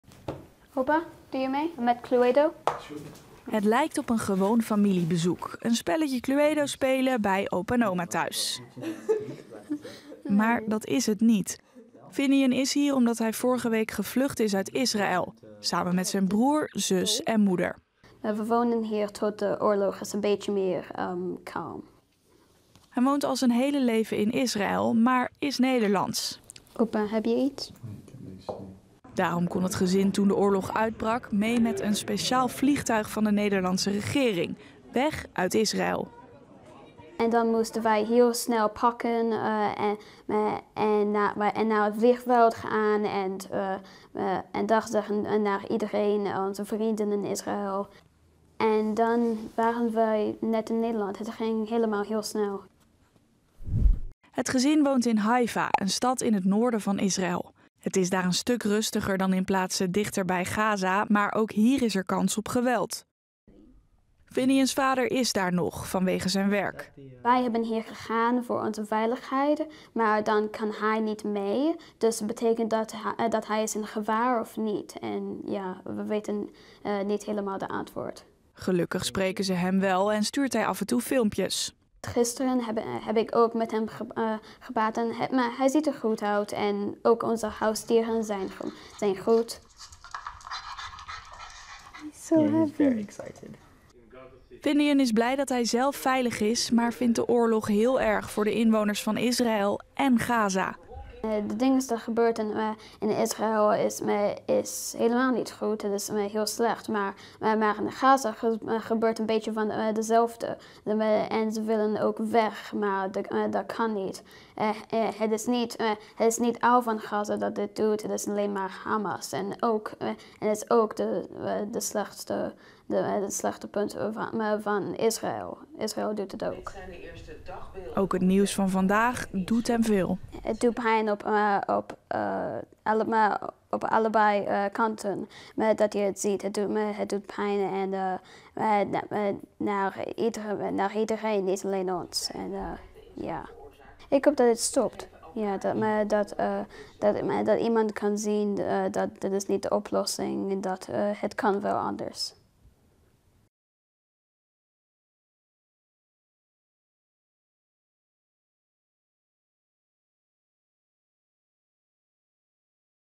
Deze kinderen zijn gevlucht uit Israël
Door de gevechten in Gaza en Israël zijn veel mensen gevlucht. Ook de 11-jarige Finnian, samen met zijn moeder, broer en zus. Ze komen uit Israël, maar wonen nu bij hun Nederlandse opa en oma.